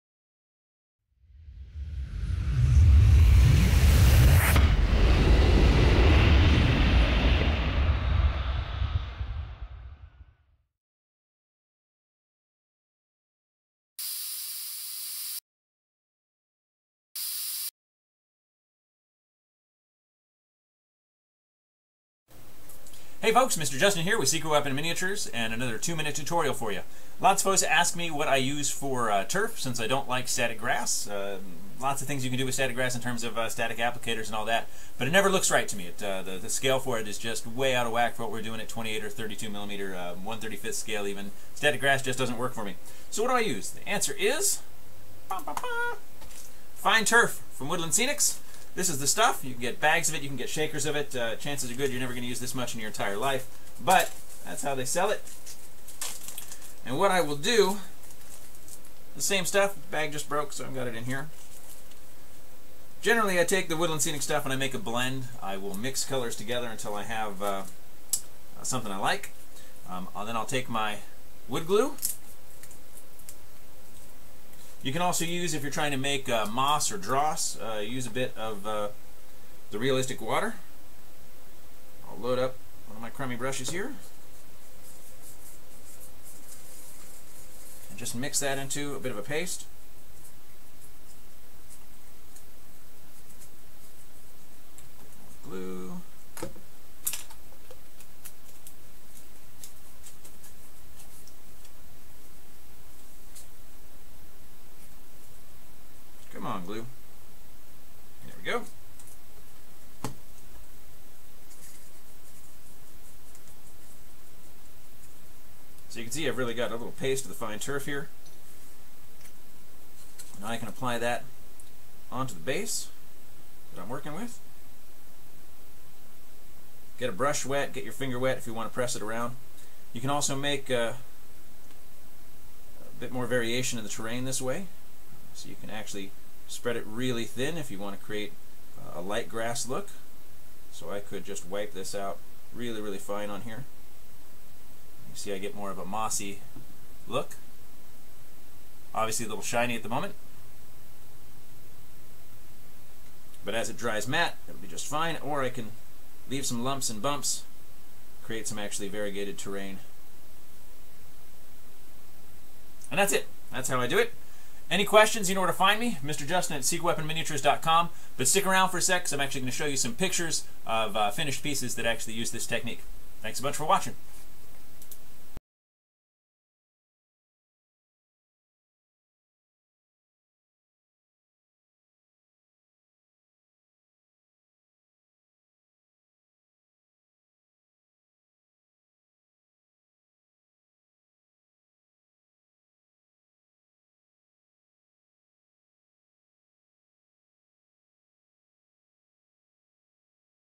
Two Minute Tutorial - Ground Flocking
A quick look at how to use fine turf for basing, in place of static grass, for a more realistic flock at scale.